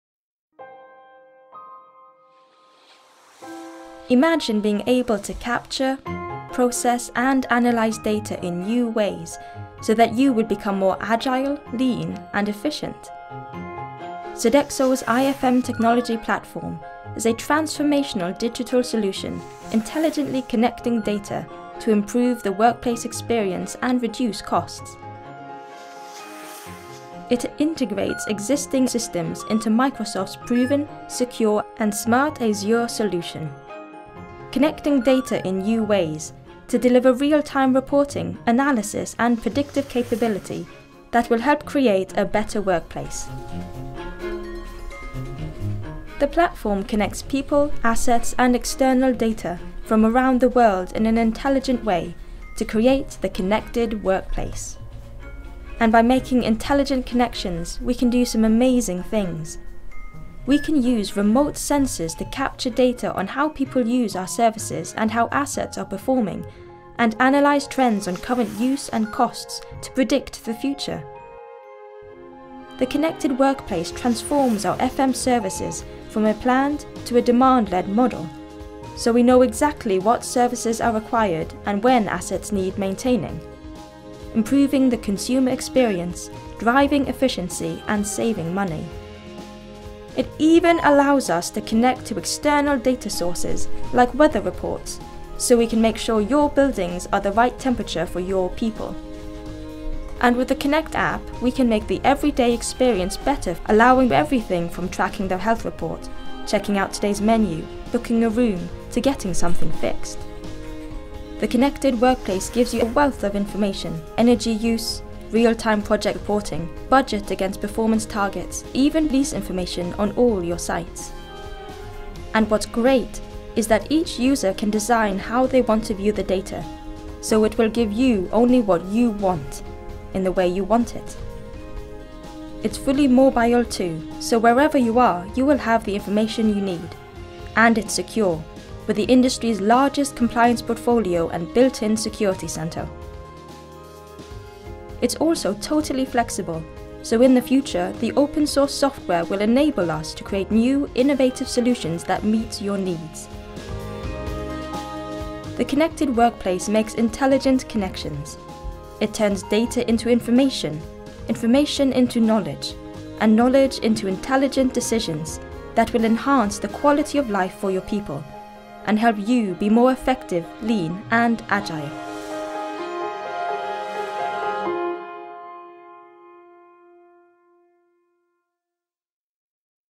IFM Technology Platform
Sodexo’s IFM Technology Platform intelligently connects people, assets and data to improve workplace experiences and reduce costs. The platform delivers real-time reporting, analysis and predictive capability on how people use our services and how assets are performing.